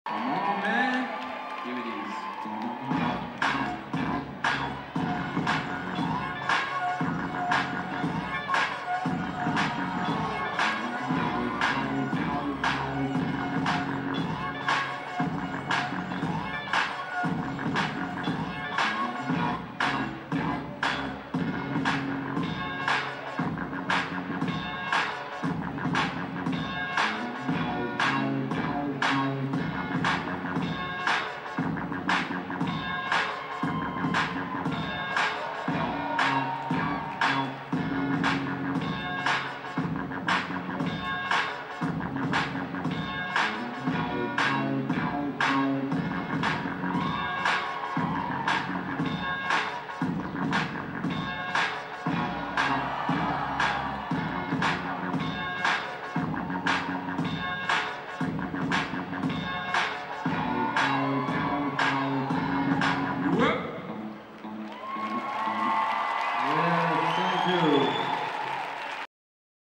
IDO World Champioship 2011 Graz - Electric Boogie - Blake Williams 1st Place
IDO World Championship 2011 Graz/Austria
Category: Electric Boogie Solo Adults - Final
Blake Williams from South Africa
1st Place